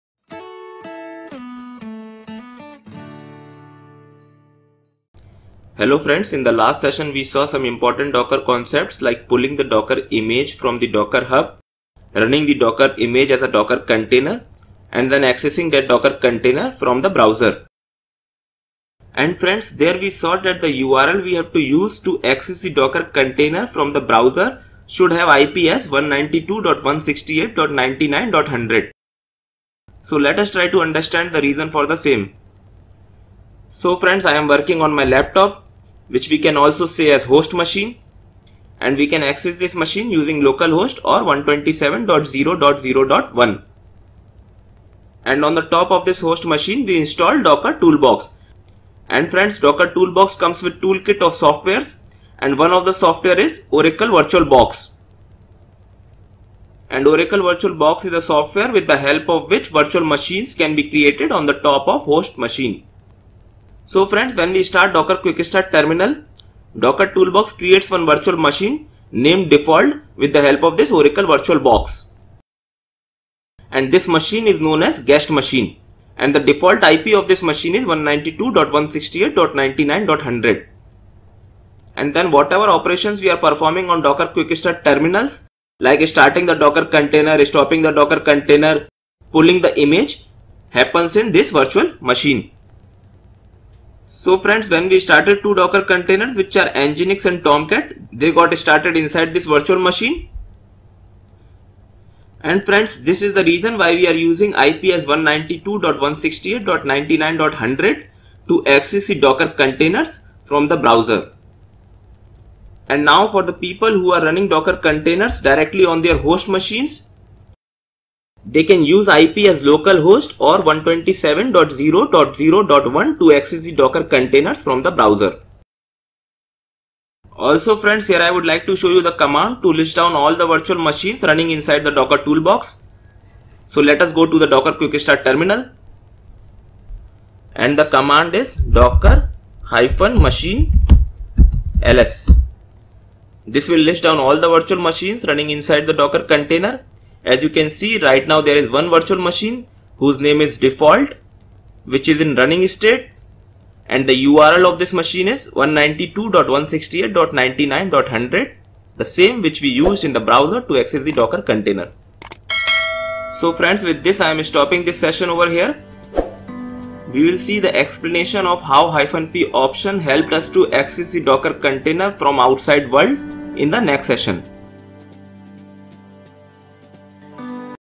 #10 VERY IMPORTANT FOR DOCKER BEGINNER | WHY IP 192.168.99.100 Used To Access Container With Toolbox
WHY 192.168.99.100 Is Used As IP Or Url To Access Docker Container With Docker Toolbox | Docker Tutorials